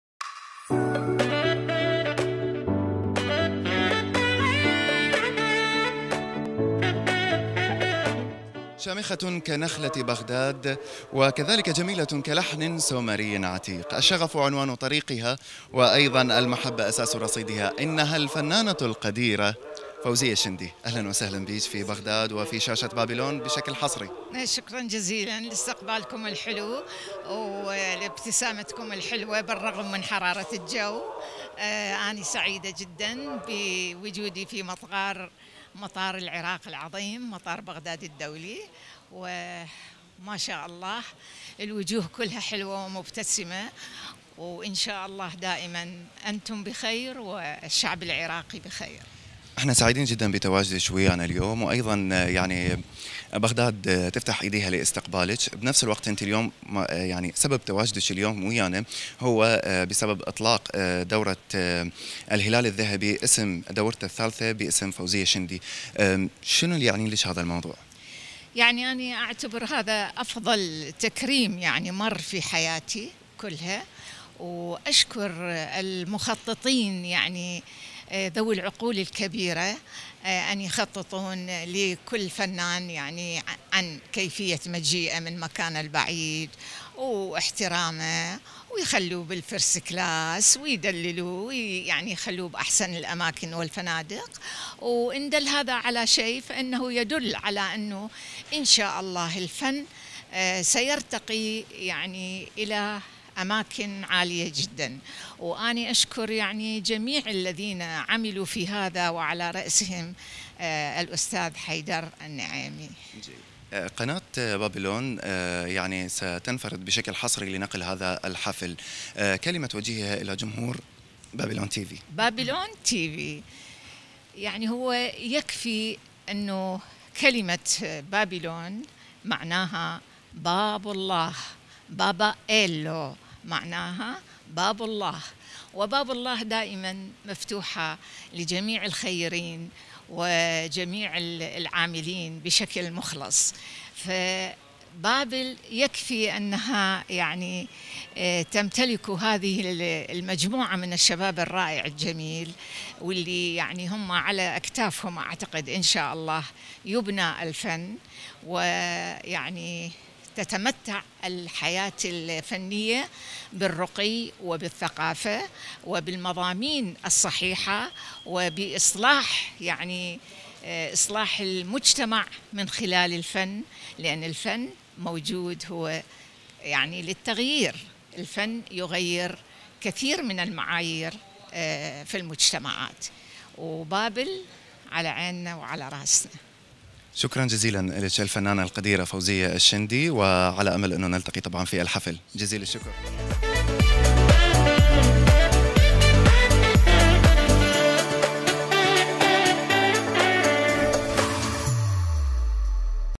استقبال الفنانة القديرة فوزية الشندي من مطار بغداد الدولي | ضمن التحضيرت لـحفل#الهلال_الذهبي
استقبال الفنانة القديرة فوزية_الشندي من مطار بغداد الدولي، ساعات قليلة تفصلنا عن حفل الهلال_الذهبي للأعمال التلفزيونية الرمضانية
نايل سات | 11637 | عمودي | 27500 | 7/8
نايل سات | 11603 | افقـــــي | 27500 | 5/6
ياه سات | 12054 | افقـــــي | 27500 | 5/6